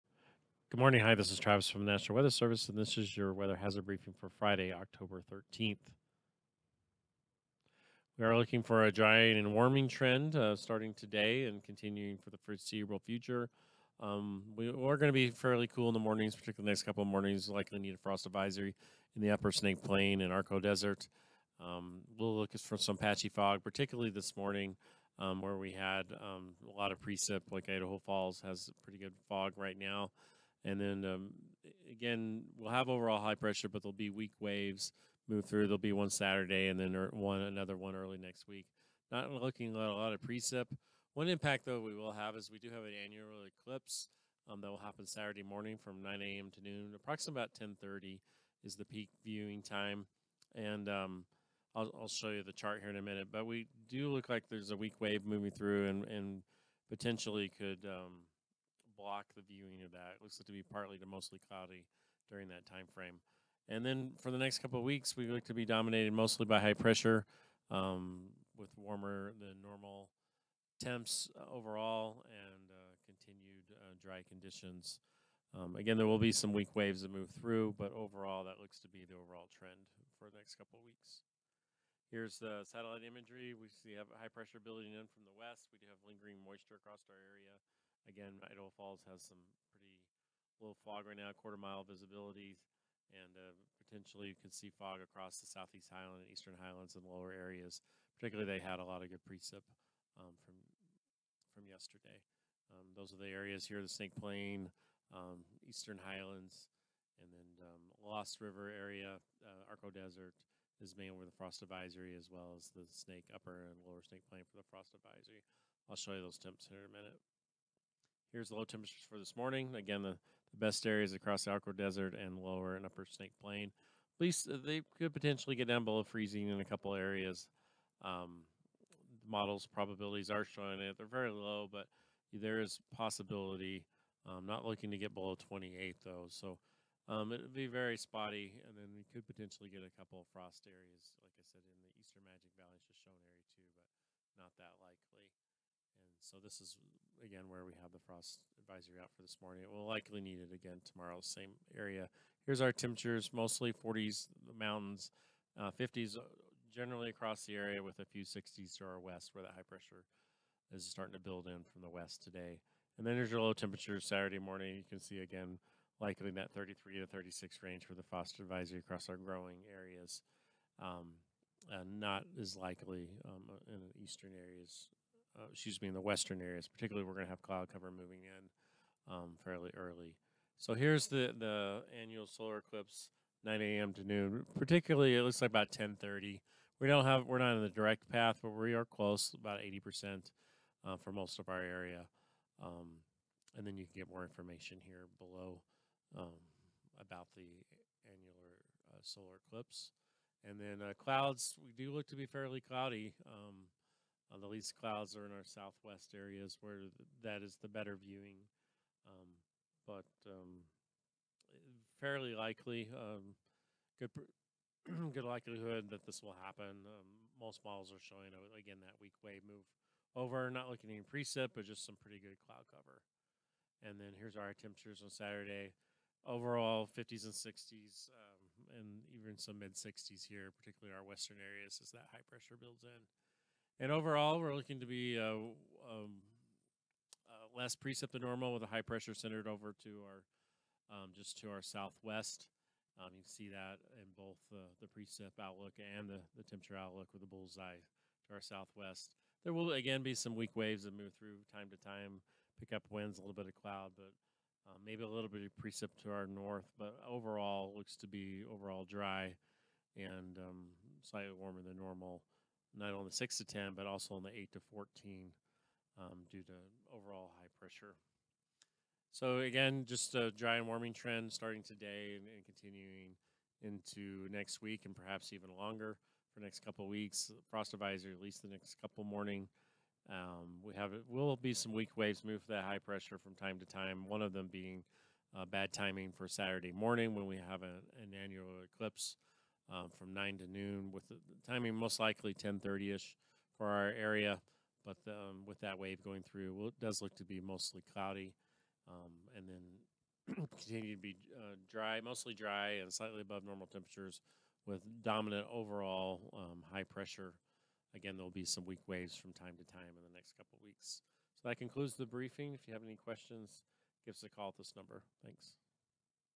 10/13/23 Hazard Briefing -Dry conditions and Warming Temperatures
for the most recent weather hazard briefing.